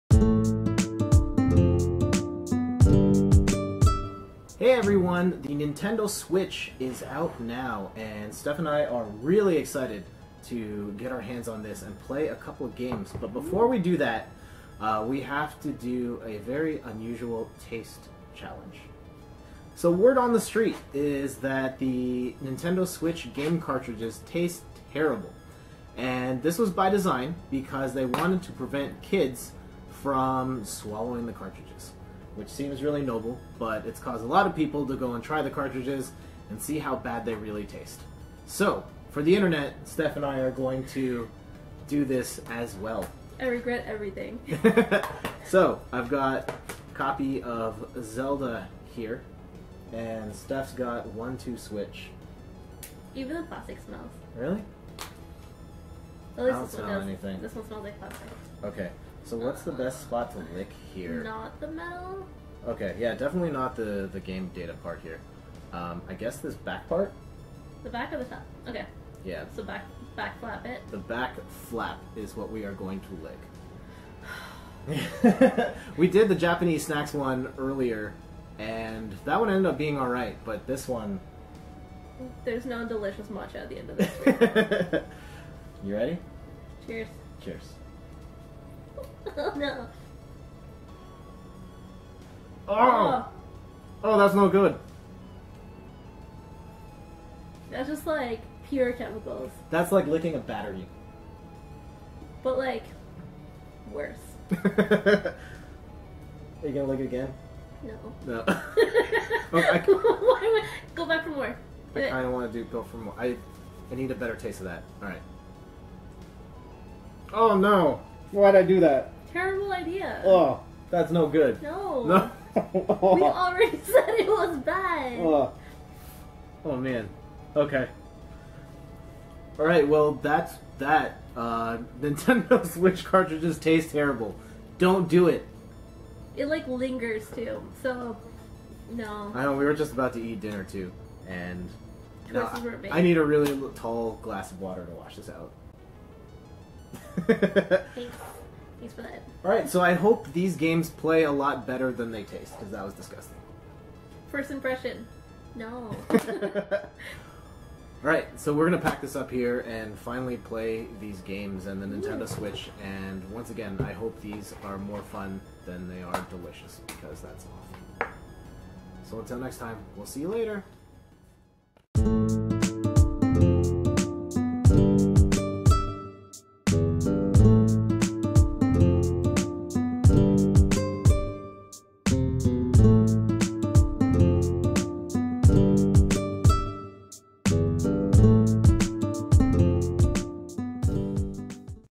Nintendo Switch Cartridge Taste Test
Word on the street is that Nintendo Switch cartridges taste terrible. We uncover the truth by licking the cartridges ourselves.